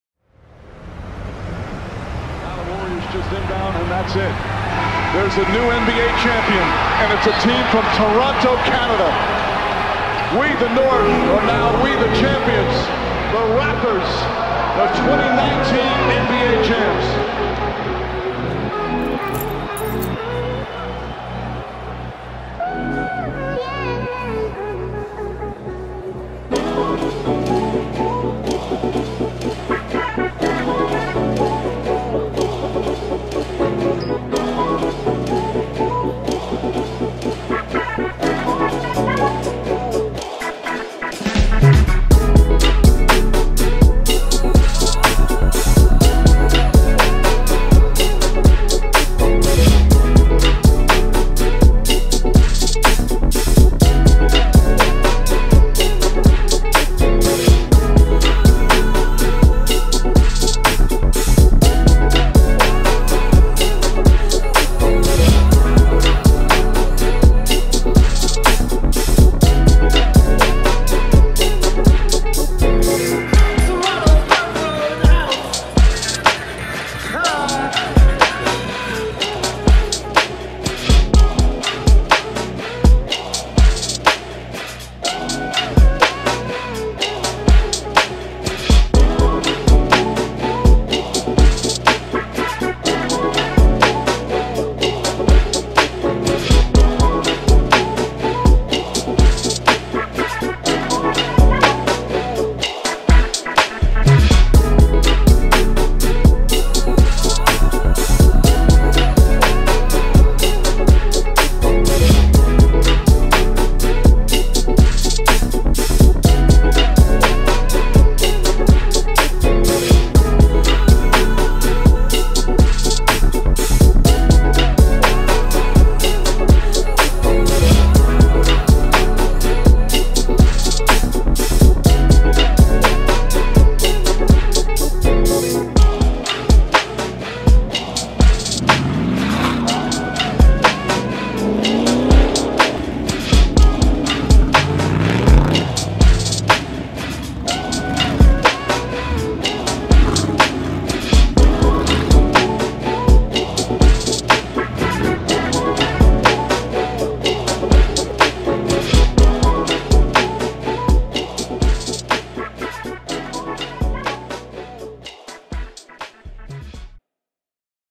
Importfest 2019 | Canada's Biggest and Baddest Car Show | Toronto
We traveled to Toronto, Ontario, for the 19th celebration of Importfest, the Biggest and Baddest Car Show in all of Canada. Vossen has been the presenting partner of the event for many years now and we are anxiously anticipating the 20th Anniversary of the event in 2020!

Follow the event at @IMPORTFEST and their performance shop at @ImportfestPerformance.